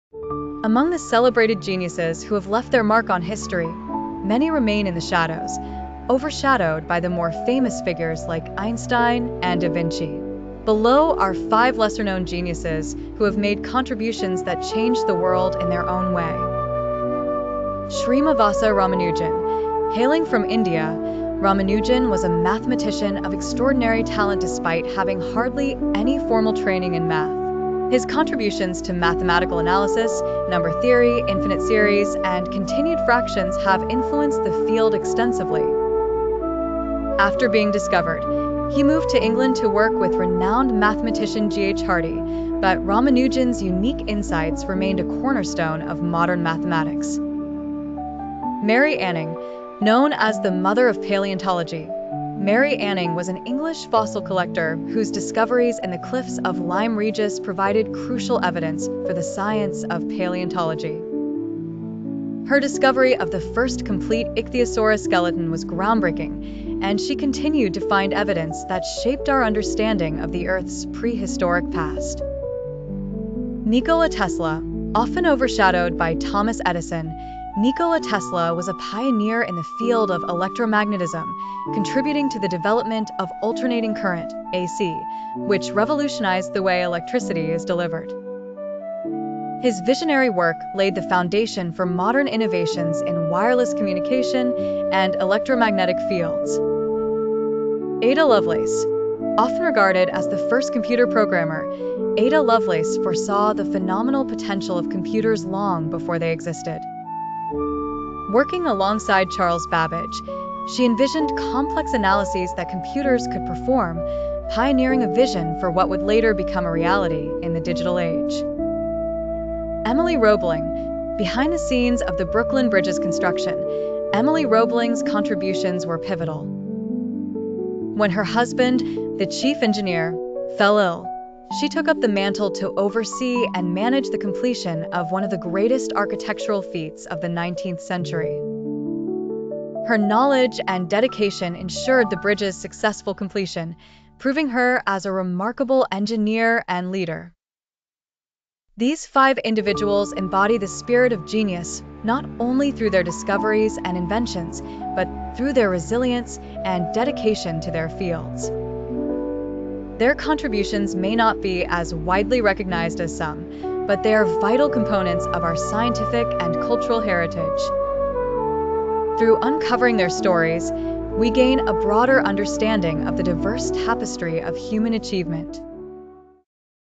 Unsung Geniuses Who Changed the World
This narrative highlights five lesser-known geniuses whose contributions significantly impacted the world. It features Srinivasa Ramanujan, a self-taught mathematician; Mary Anning, the pioneering fossil collector; Nikola Tesla, the visionary behind alternating current; Ada Lovelace, the first computer programmer; and Emily Roebling, who played a crucial role in the Brooklyn Bridge's construction. Their stories reveal the importance of resilience and dedication in shaping our scientific and cultural heritage.